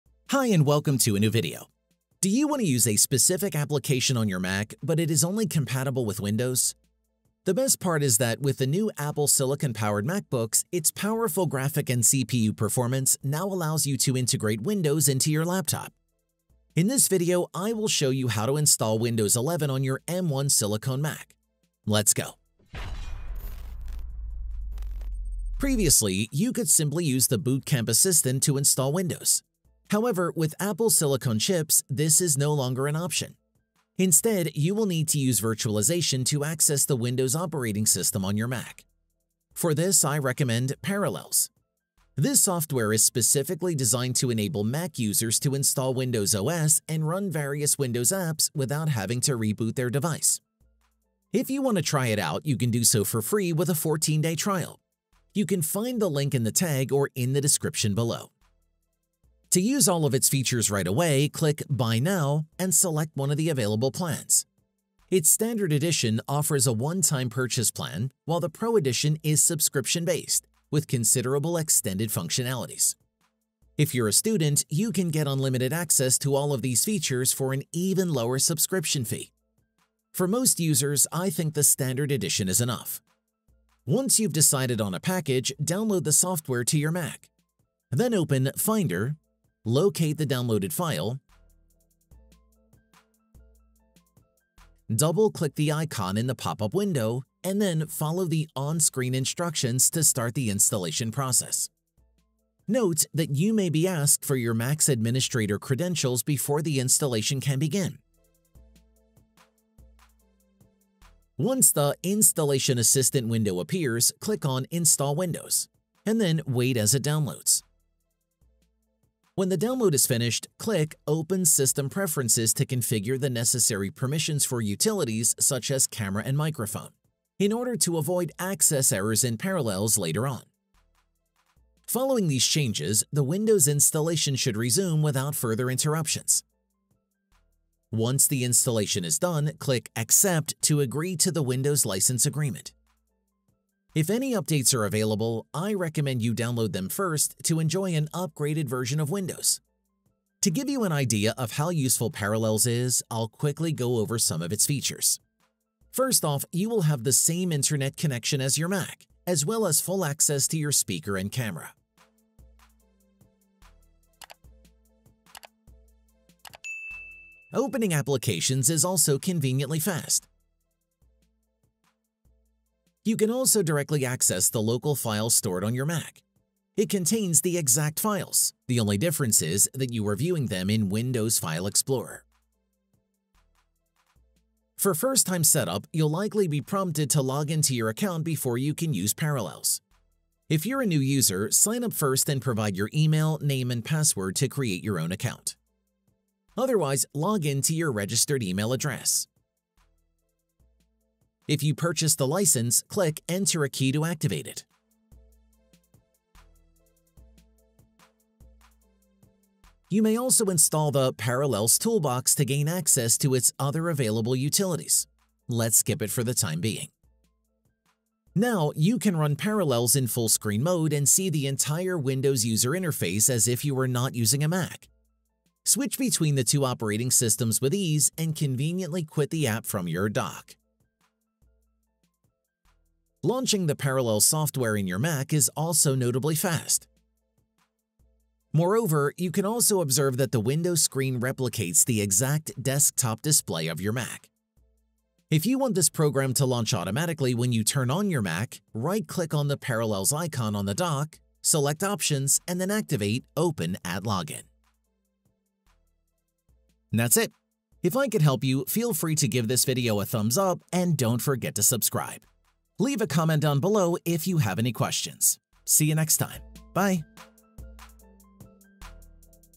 How to Install Windows 11 on M1 or M2 Apple Silicon MacBook! [2023]
Previously with Bootcamp Assistant, you can easily use Windows on your Mac. With Apple's new Silicon-powered MacBooks, this is no longer an option. But of course, there is always an alternative! In this video, I will show you how to install Windows 11 on your M1 Silicon Mac. Yours Jigxor

Most important links:
* | Download Parallels

Timestamps:
00:00 | Download and Install Parallels
02:17 | Install Windows
03:04 | Setup Parallels

Video: 201